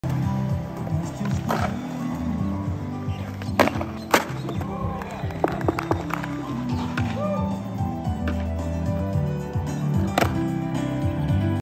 180 Nose Grind at 9:30am - 49 years old
9:30am this morning - 22nd Jan, 2023 - Birkenhead Skatepark, Auckland, NZ.
Aged 49 and a quarter.